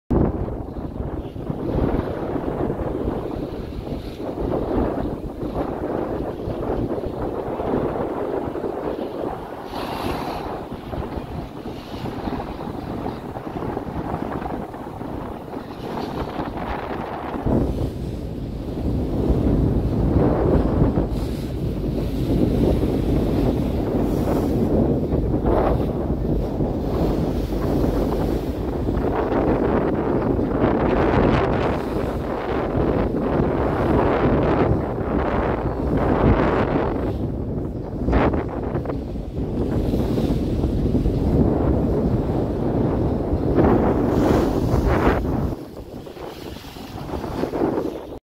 Red Sea / Paradise Island, Hurghada, Egypt
Paradise Island is a part of the Giftun island, it’s a small piece of a real Paradise just off the Hurghada coast.